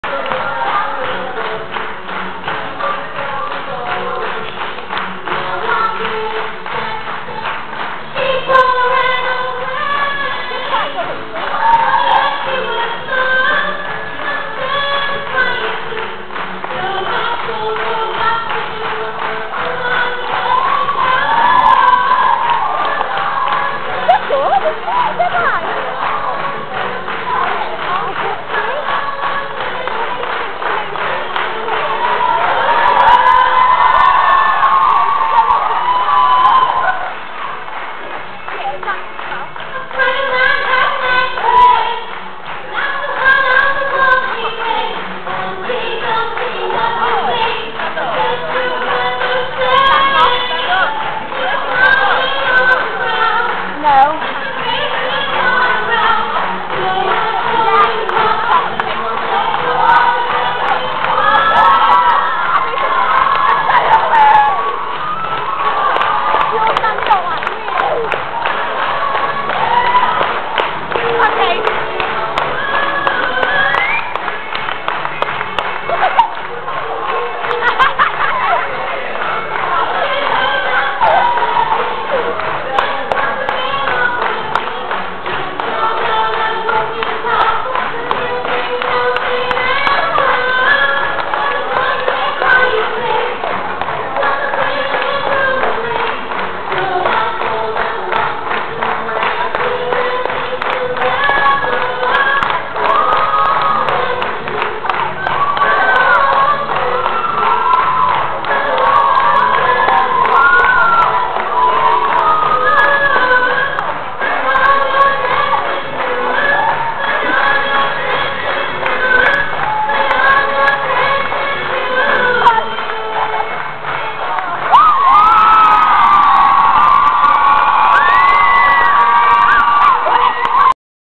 trenthams got talent
girl singing... ACE!!! singer and dancer! x